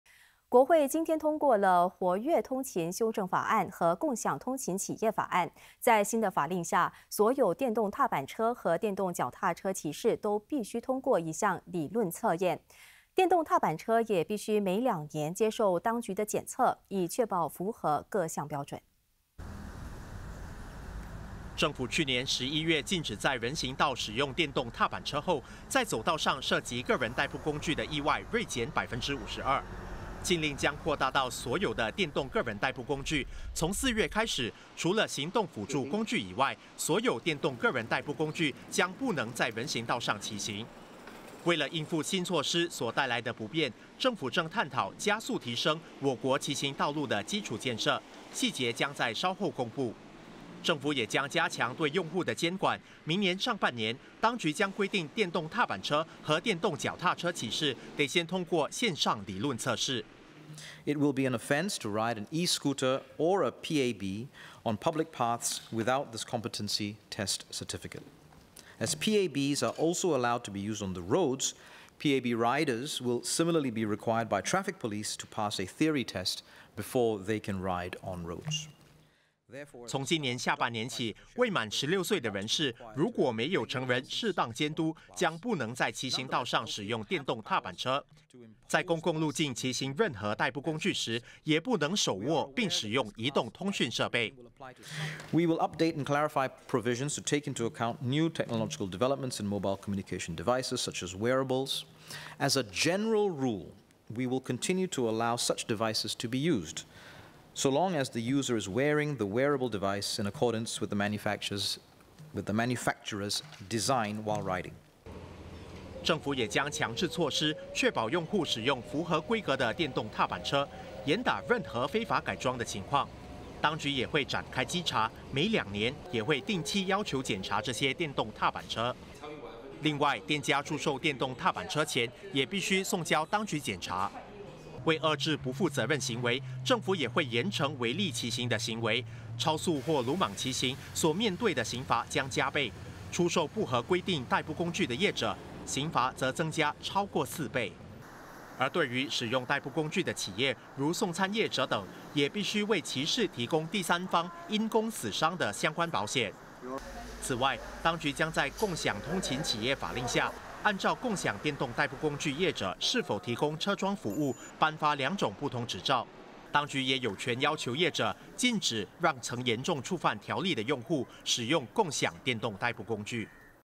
【国会】《活跃通勤修正法案》加以监管代步工具用户和企业 确保安全
在新法令下，所有电动踏板车和电动脚踏车骑士都必须通过一项理论测验。电动踏板车也必须每两年接受当局的检测，以确保符合各项标准。